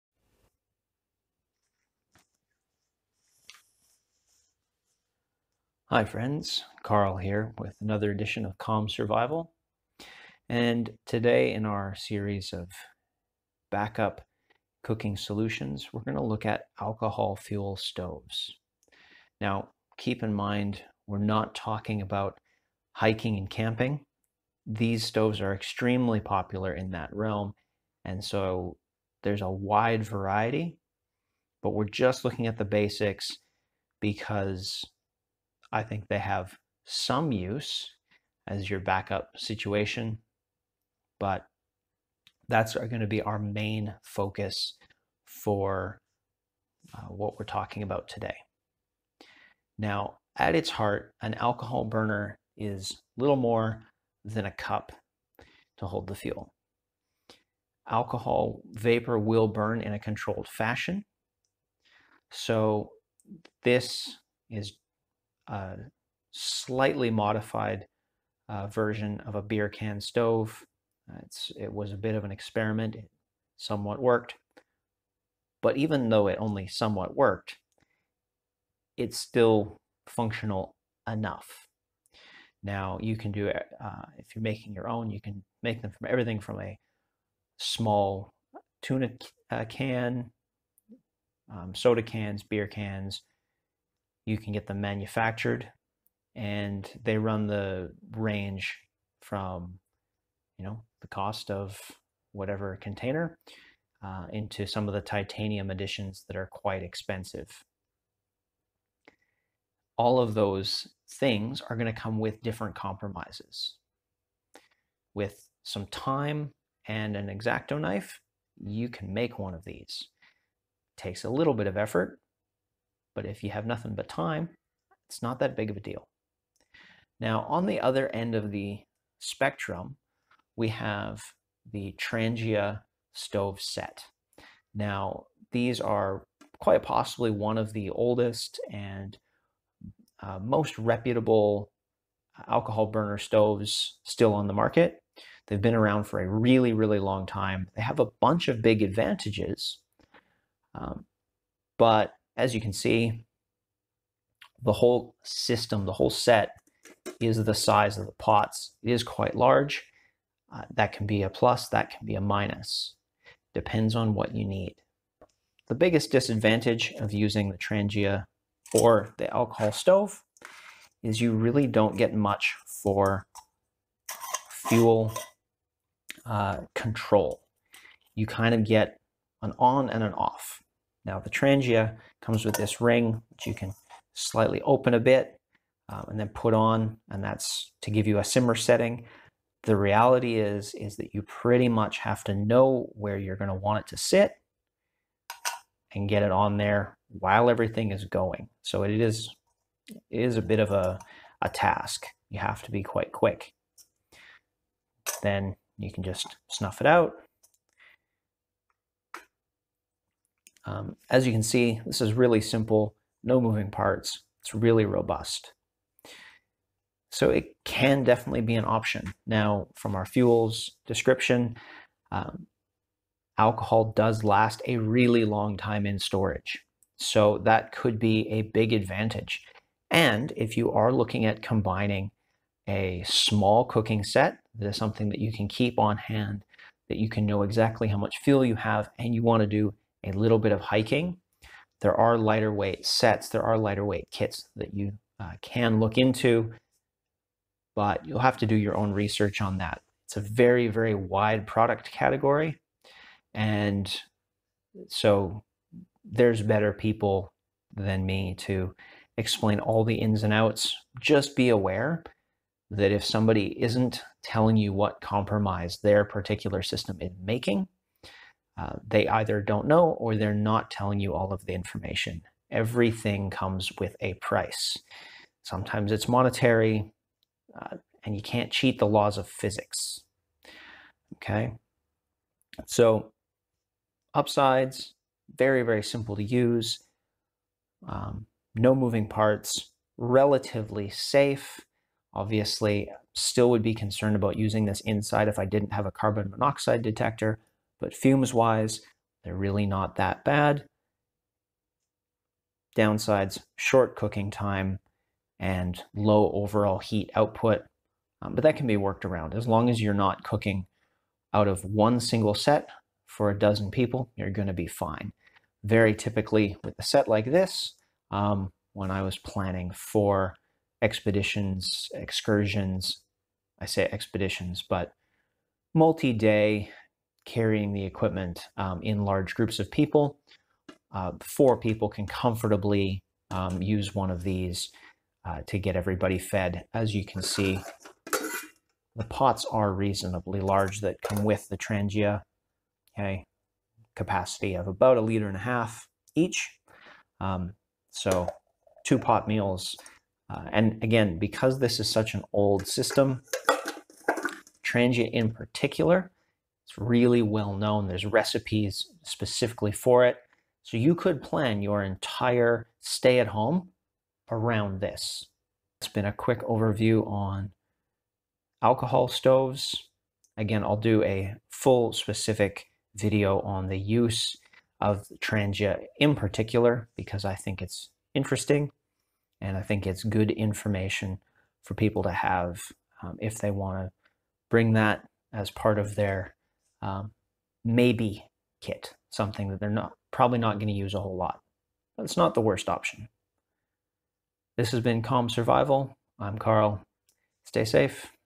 Alcohol Stove basics
Today we are looking at a couple varieties of alcohol stoves, and how they might fit into your home backup plan. Because of the overall simplicity of the stoves, I do focus a lot on the Trangia, but there is a lot to be applied to other systems.